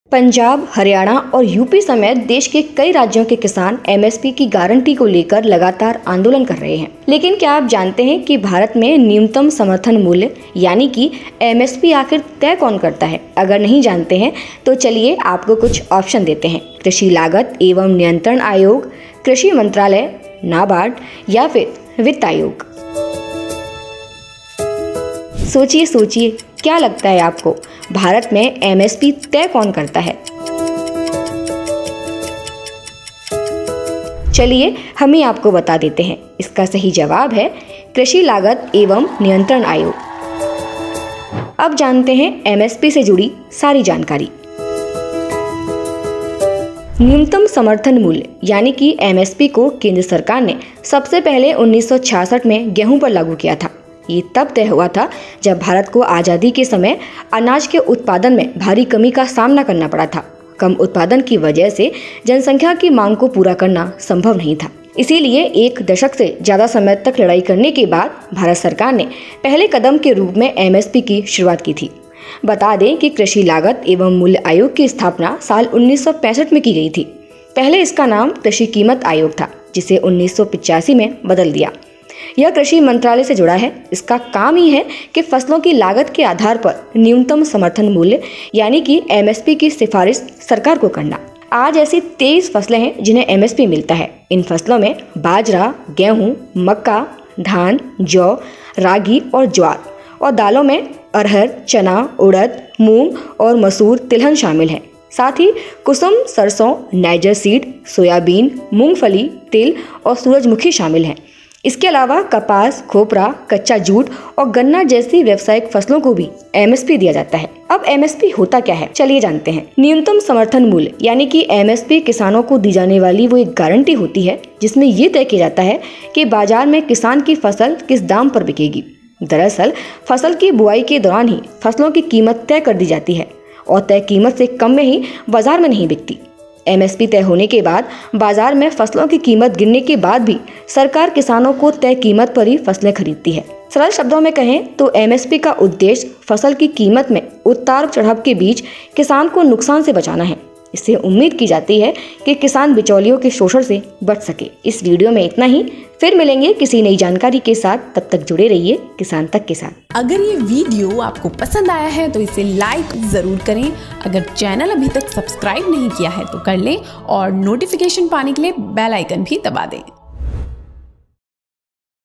Agri Quiz: क्या आप जानते हैं India में MSP कौन तय करता है? agriculture | Kisan Tak
पंजाब, हरियाणा और उत्तर प्रदेश समेत देश के कई राज्यों के किसान  MSP की गारंटी को लेकर लगातार आंदोलन कर रहे हैं.लेकिन क्या आप जानते हैं..भारत में न्यूनतम समर्थन मूल्य, यानी MSP, आखिर कौन तय करता है?